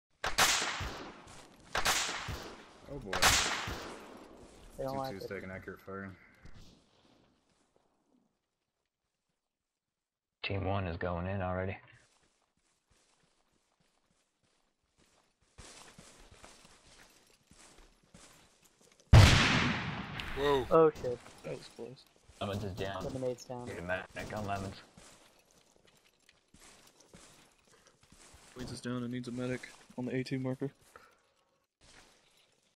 Lemon's 'Nade - Bourbon Warfare TVT
During the search for the Golden AK TVT mission, our fireteam started taking pretty accurate fire.